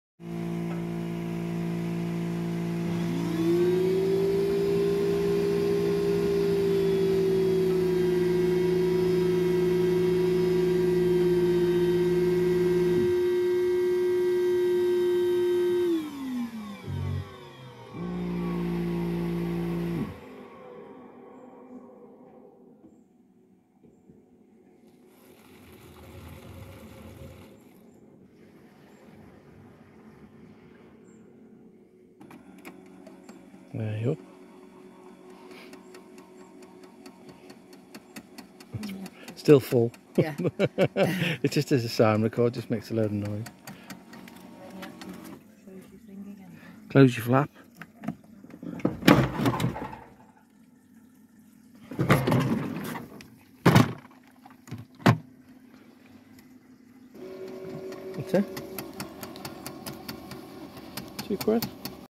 Camperclean cassette toilet cleaner
Made all the right noises, not sure it added enough chemical for our liking, £2 we only pee in ours

But if you don't want splashback doing it in a disposal and your not doing it 3 times a day worth a try.